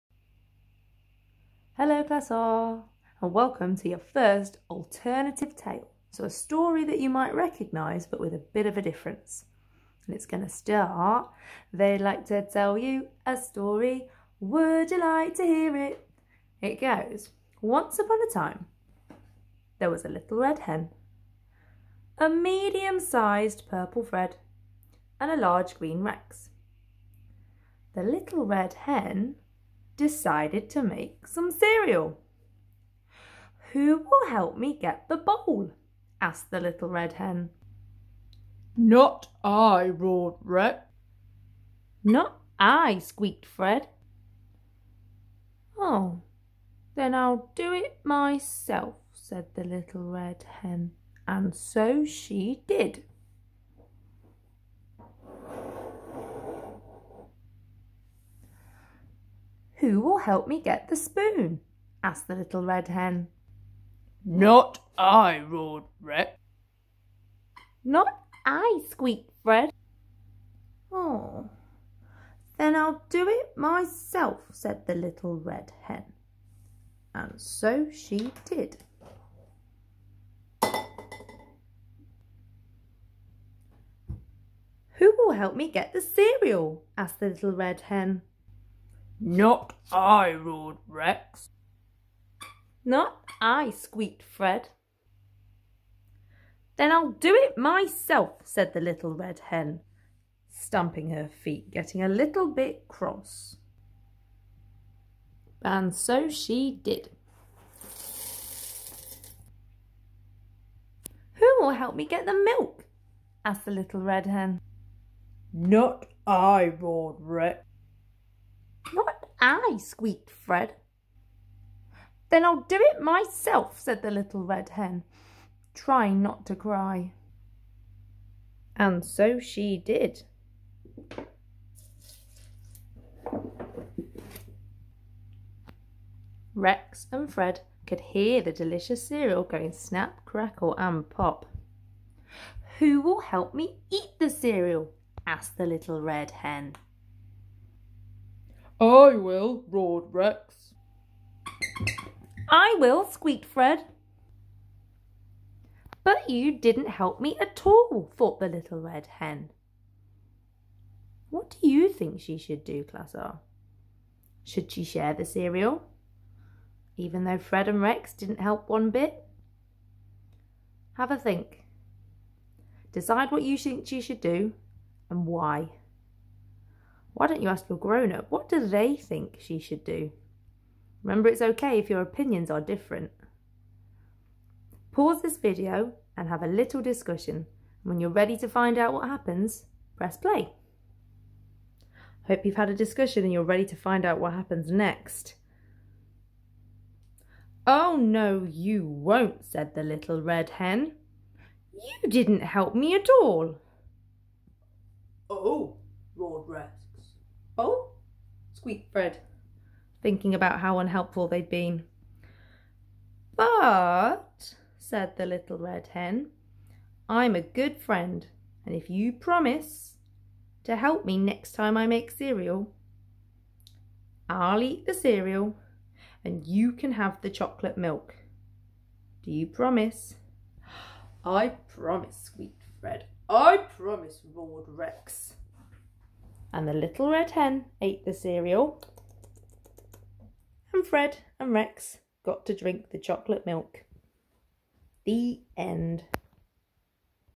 Mrs Bird's Home Learning 🐦🏡 The Little Red Hen Makes Cereal ~ Alternative Tale 📖
Can you spot what's the same as The Little Red Hen story?
Can you spot what's different from The Little Red Hen story?
Can you make up your own alternative tale?